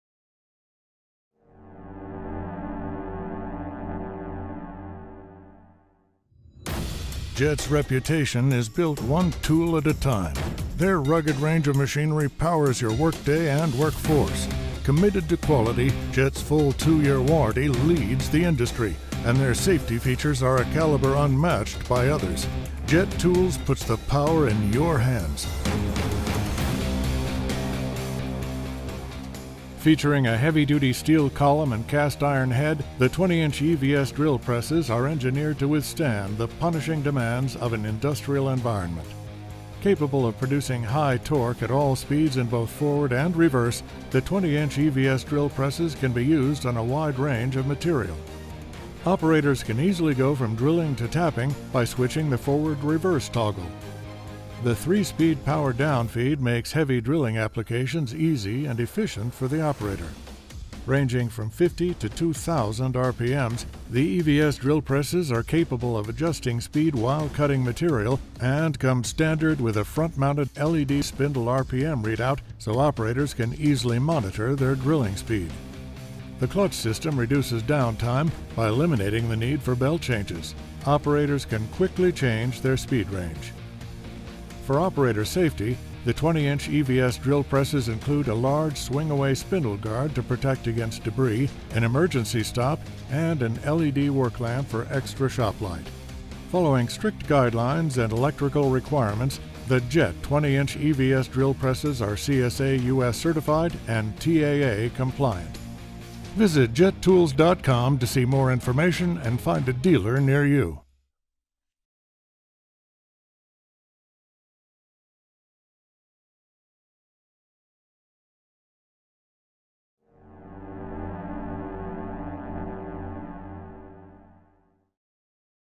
JET 20-Inch EVS Metal Drill Presses
The 20'' JET JDPE-20EVS-PDF EVS drill press produces high torque at all speeds in both forward and reverse necessary for a wide range of operations. Engineered to maximize output the 20’’ EVS drill press includes a three speed power down feed for heavy drilling applications and is capable of running from 50 to 2000 RPMs.